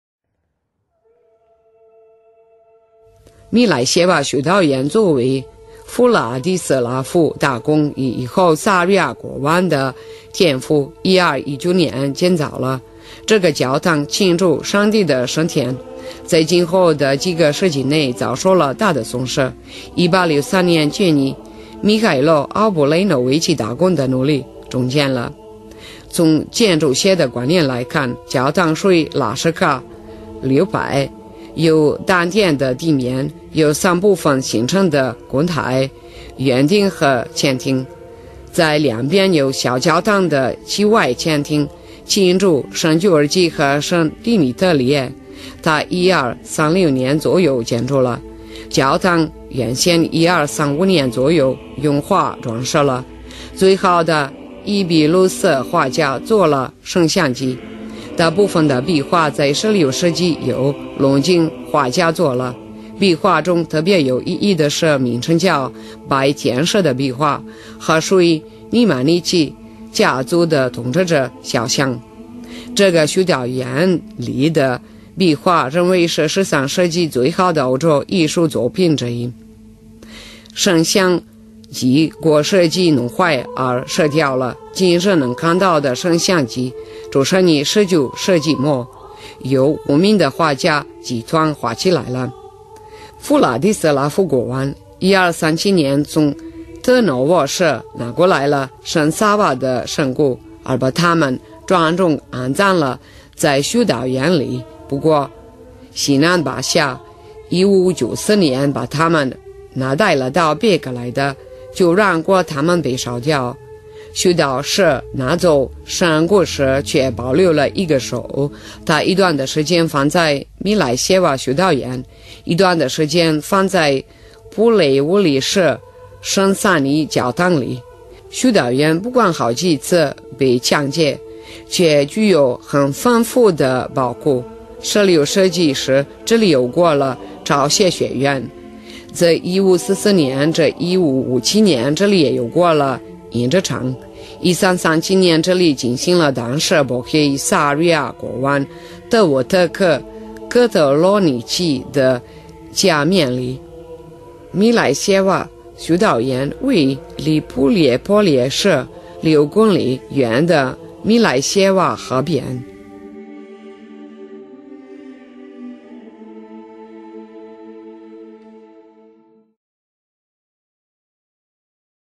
米莱谢瓦修道院
米莱谢瓦修道院作为弗拉蒂斯拉夫大公与以后塞尔维亚国王的天赋1219年建造了。这个教堂庆祝上帝的升天，在今后的几个世纪内遭受了大的损失，1863年鉴于米哈伊洛-奥布雷诺维奇大公的努力重建了。